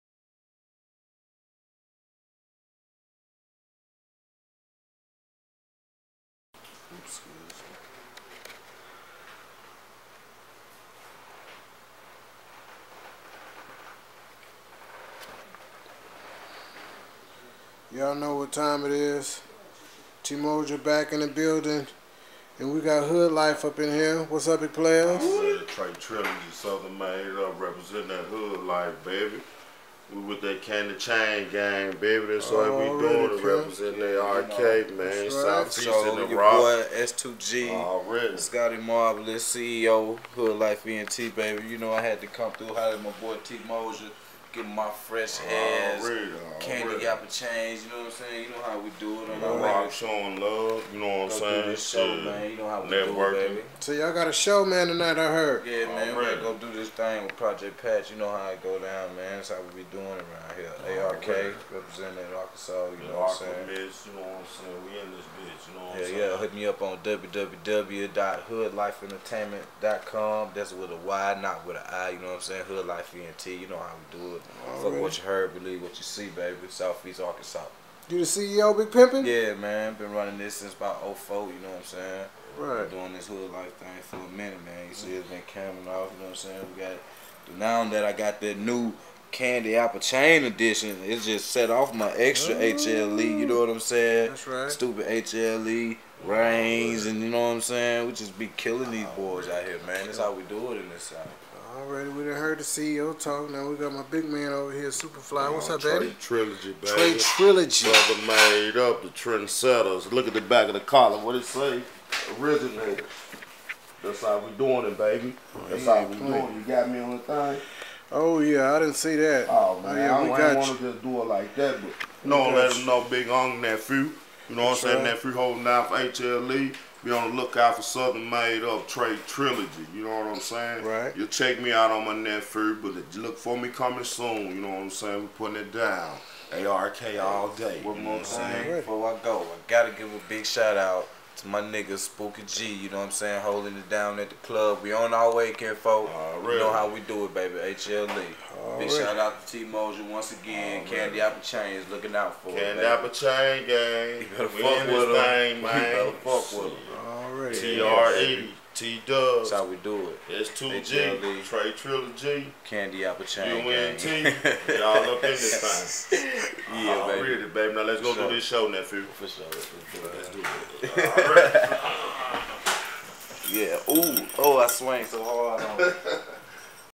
HOOD LYFE ENT. GETS THERE CANDY APPLE CHAINS
HOOD LYFE ENT, GET THERE CANDY APPLE CHAINS AND A QUICK INTERVIEW WITH TEMOLJA THE CANDY MAN!!